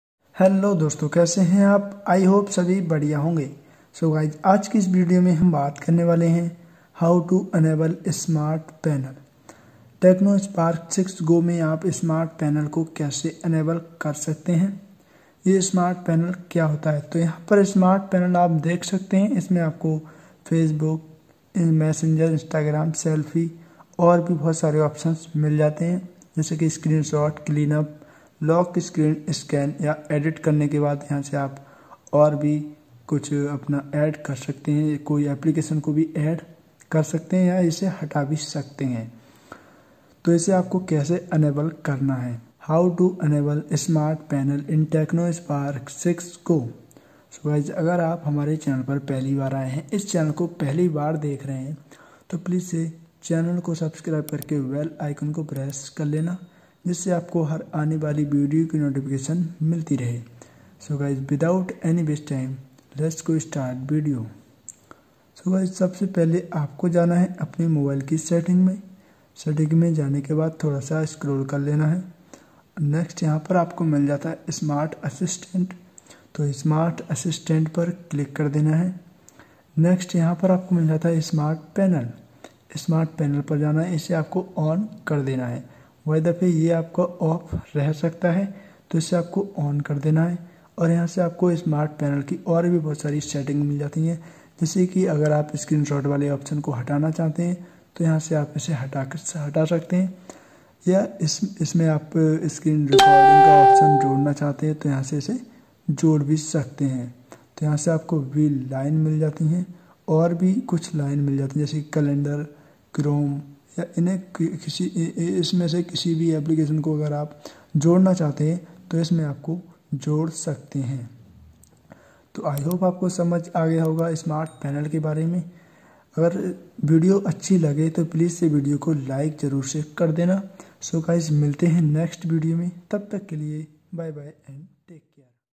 Tecno Spark 6 Go Smart Penal किया होता है | How to enable Smart penal bin Tecno Spark 6go| AksTech4u
Hello Friends

‌ Tecno Spark 6 Go All Videos.

2. How to Enable dark mode.